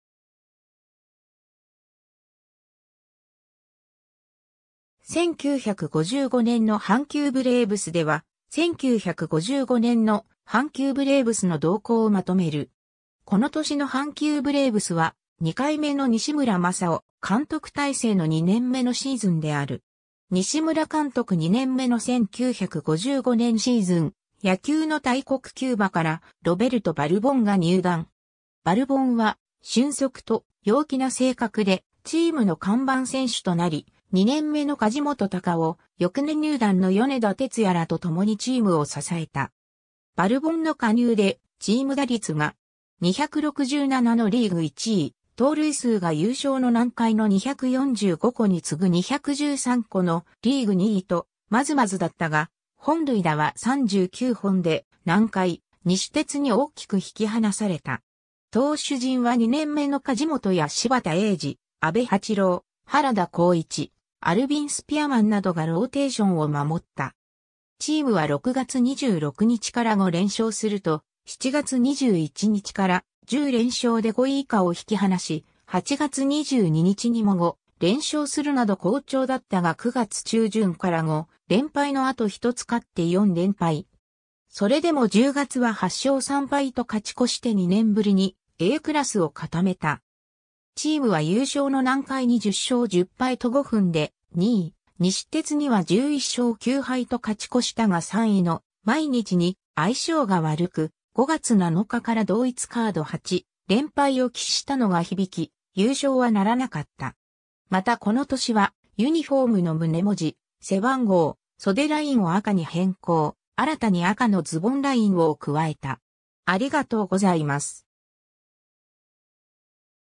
1955年の阪急ブレーブス
各年のオリックス・バファローズ
1955年の阪急ブレーブスでは、1955年の阪急ブレーブスの動向をまとめる。
 この年の阪急ブレーブスは、2回目の西村正夫監督体制の2年目（通算6年目）のシーズンである。
 西村監督2年目の1955年シーズン、野球の大国キューバからロベルト・バルボンが入団。
バルボンは俊足と陽気な性格でチームの看板選手となり、2年目の梶本隆夫、翌年入団の米田哲也らとともにチームを支えた。
バルボンの加入でチーム打率が.267のリーグ1位、盗塁数が優勝の南海の245個に次ぐ213個のリーグ2位とまずまずだったが、本塁打は39本で南海・西鉄に大きく引き離された。
投手陣は2年目の梶本や柴田英治、阿部八郎、原田孝一、アルビン・スピアマンなどがローテーションを守った。
チームは6月26日から5連勝すると、7月21日から10連勝で5位以下を引き離し、8月22日にも5連勝するなど好調だったが9月中旬から5連敗の後1つ勝って4連敗。
それでも10月は8勝3敗と勝ち越して2年ぶりにAクラスを固めた。
チームは優勝の南海に10勝10敗と五分で、2位西鉄には11勝9敗と勝ち越したが3位の毎日に相性が悪く、5月7日から同一カード8連敗を喫したのが響き、優勝はならなかった。
またこの年は、ユニフォームの胸文字・背番号・袖ラインを赤に変更、新たに赤のズボンラインを加えた